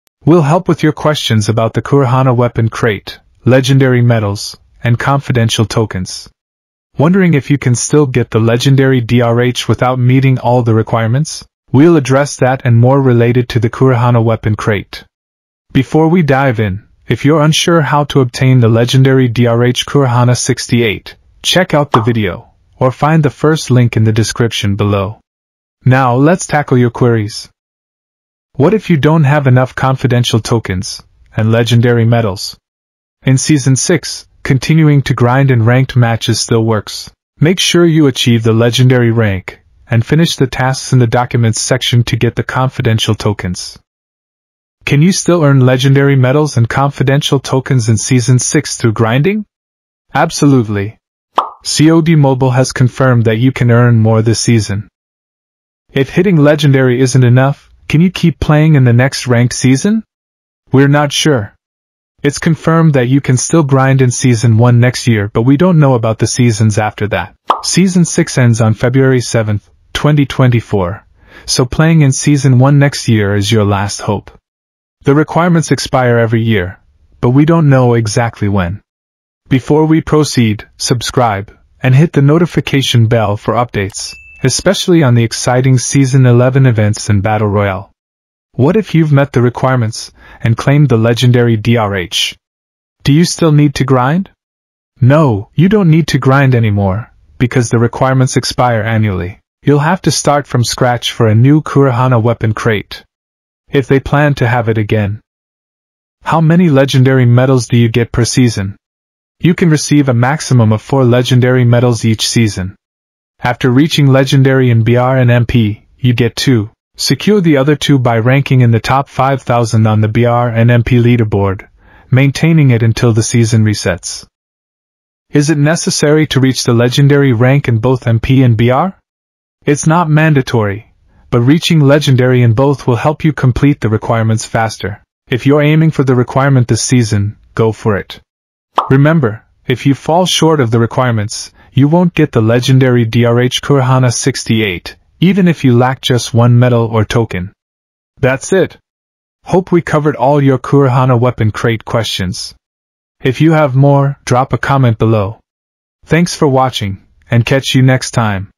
CAN I STILL GET FREE DR-H - KUROHARA - 68 IN SEASON 6?
Get insights on grinding in Season 6 and find out if you can continue in the next ranked season. Don't miss this guide for all your burning questions!

💬 Join the Chat:
Share thoughts and questions in the comments.